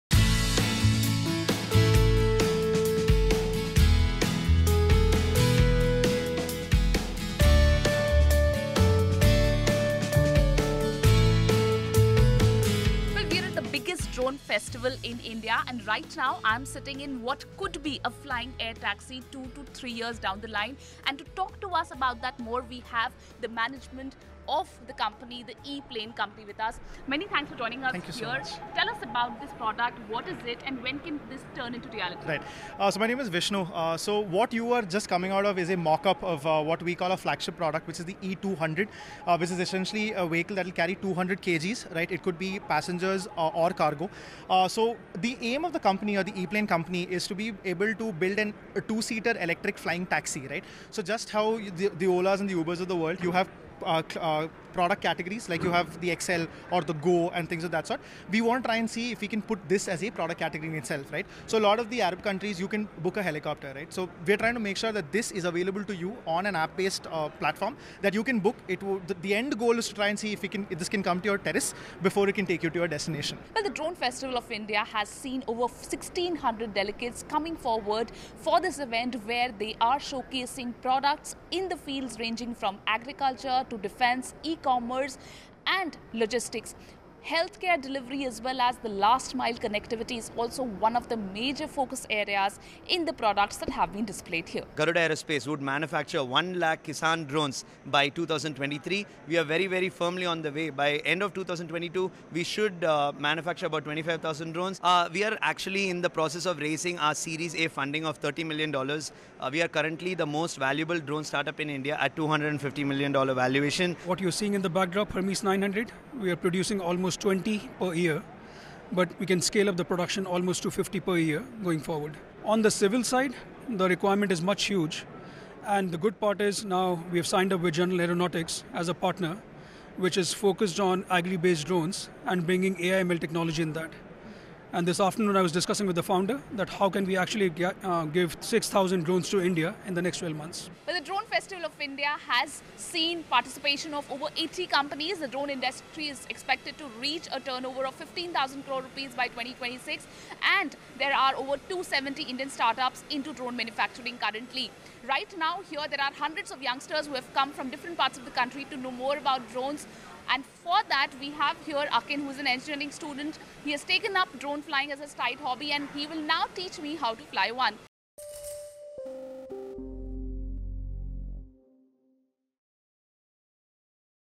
India's Biggest Drone Festival
PM Narendra Modi inaugurated the India's biggest drone festival in Delhi. Anu Sharma takes you inside the expo for a sneak peek.